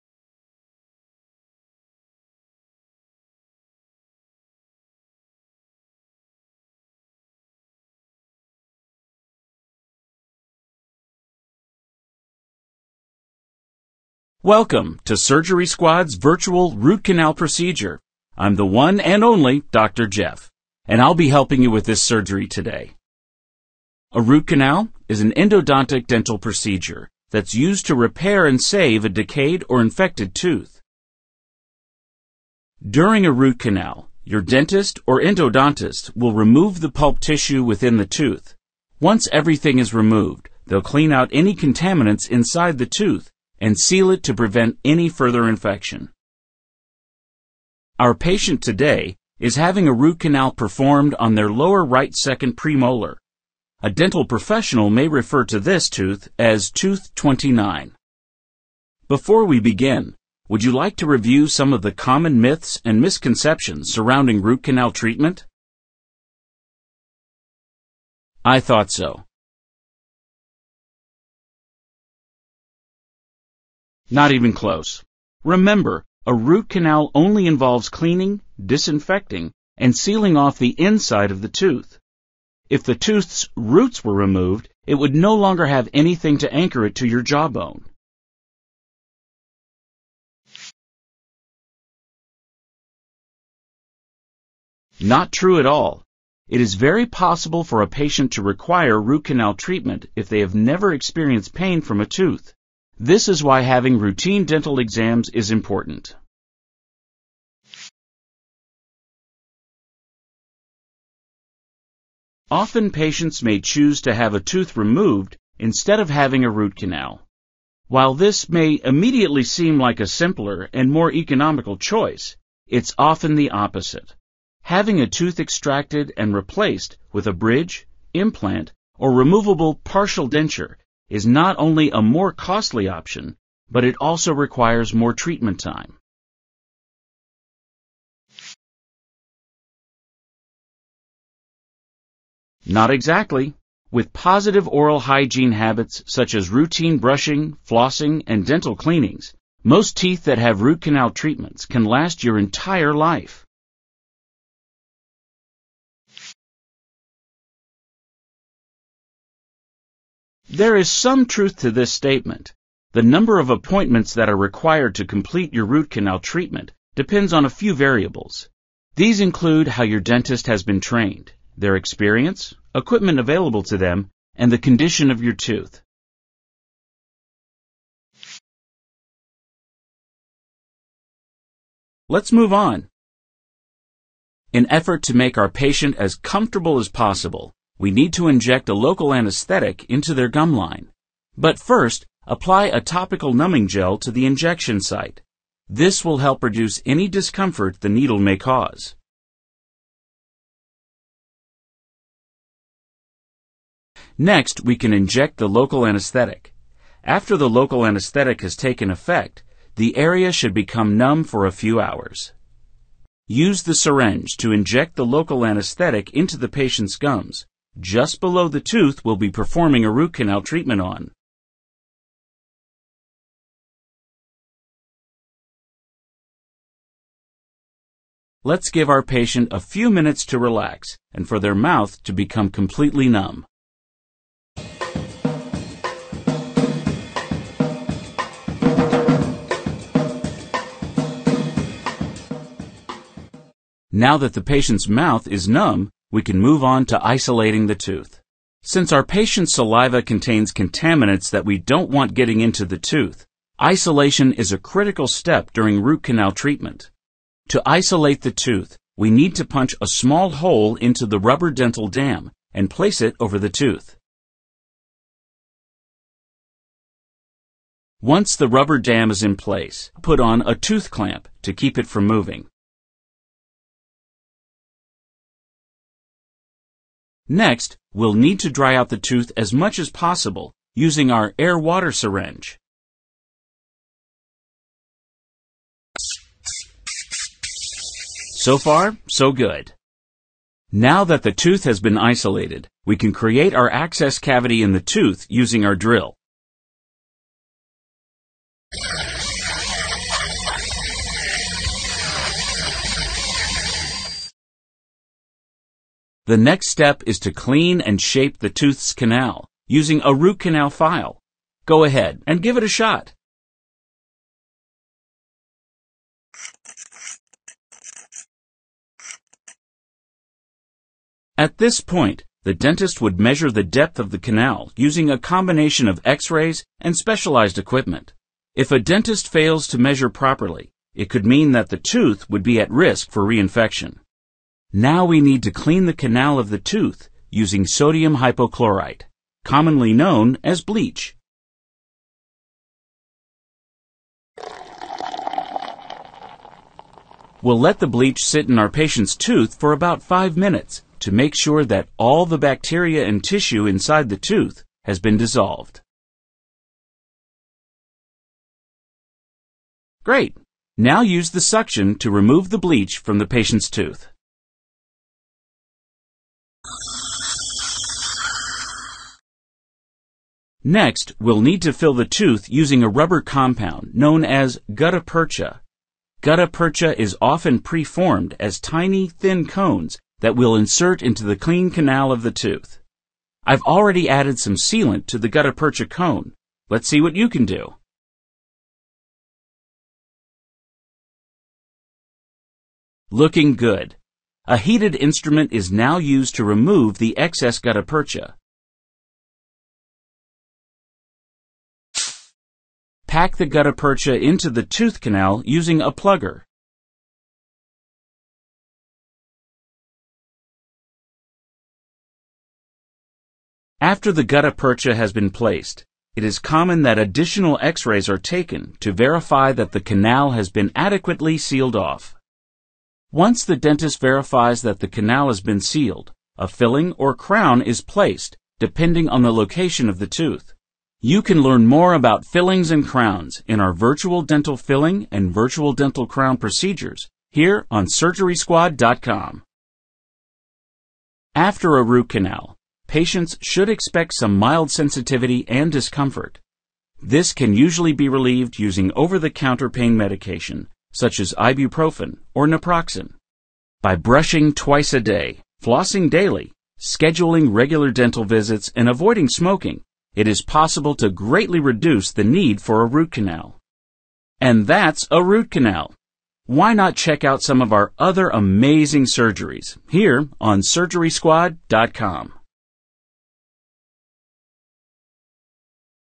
Root Canal Surgery
What could be worse than a root canal?  A lot of things! The root canal is probably one of the most misunderstood dental procedures performed today, and is thought to be incredibly painful with lasting side effects. Not true!  A root canal is an endodontic procedure used to repair and save an abscessed or decayed tooth. During this interactive root canal, you will isolate the tooth and clean the tooth's root canal with a special file.  After irrigating the area to clean it thoroughly, you will fill the cavity with a rubber compound known as gutta-percha and seal it off. When finished, you will learn how to avoid tooth decay and dental disease in the future. That doesn't sound bad, does it? Scrub in and let's get started!